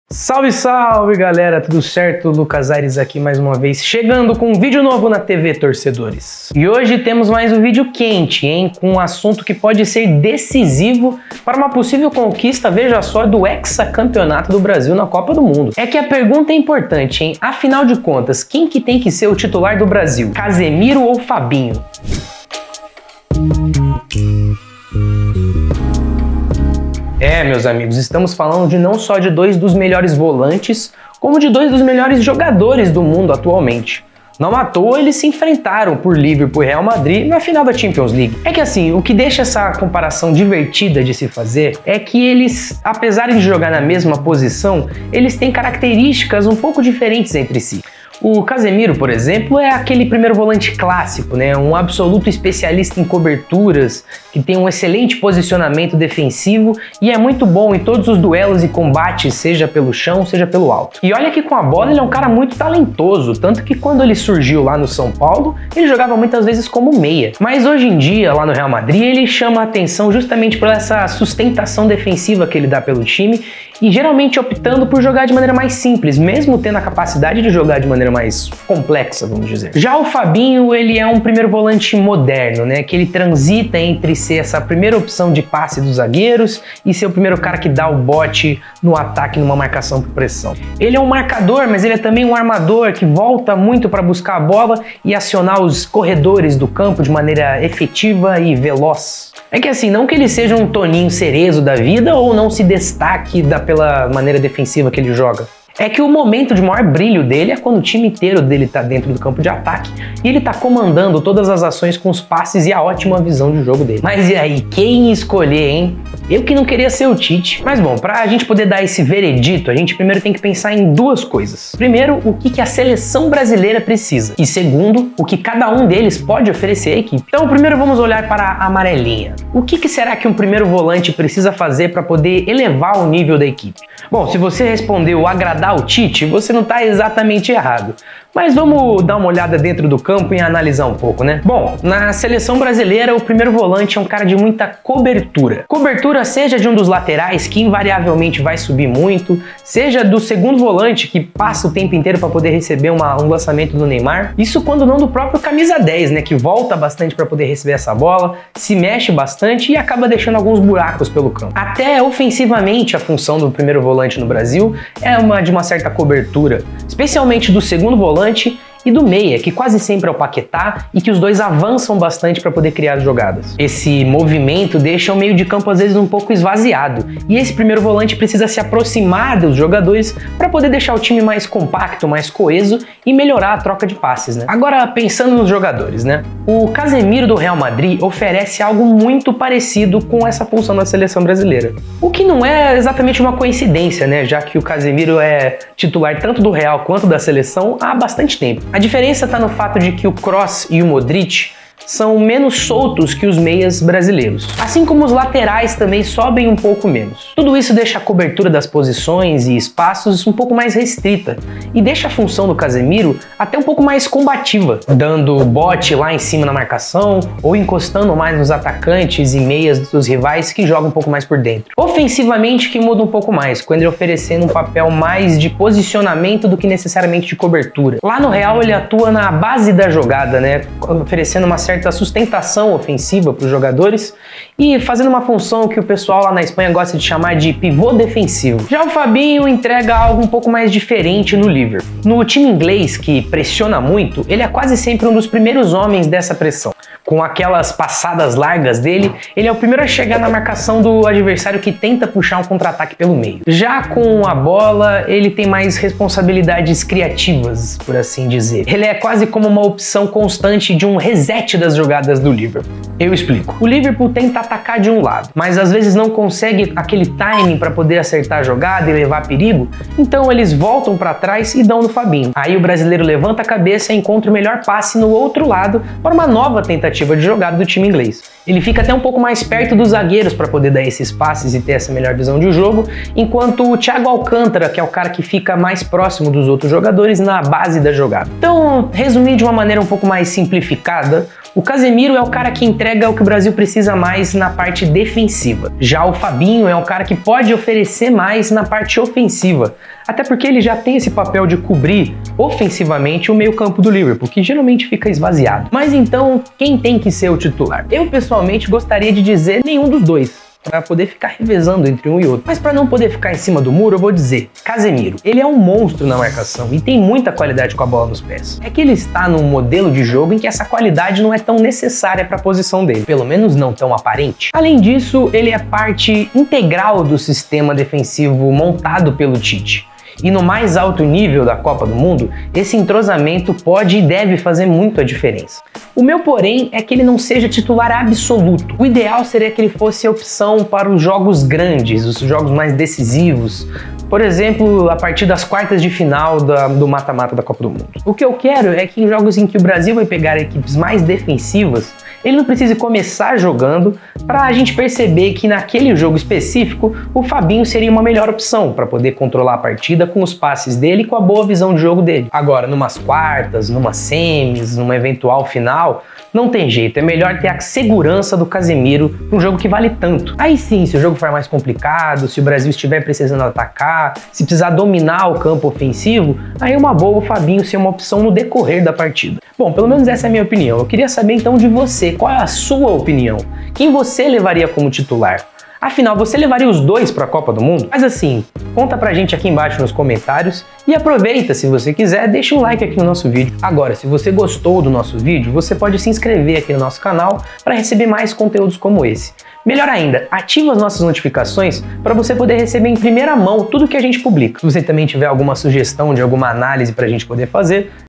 Casemiro x Fabinho: Quem deve ser o titular na Copa do Mundo?
A Copa do Mundo de 2022 está chegando e o técnico Tite, apesar de estar avançado na equipe titular da seleção brasileira, ainda paira uma dúvida: qual deve ser o volante, Casemiro, do Real Madrid, o Fabinho do Liverpool? Veja a nossa análise!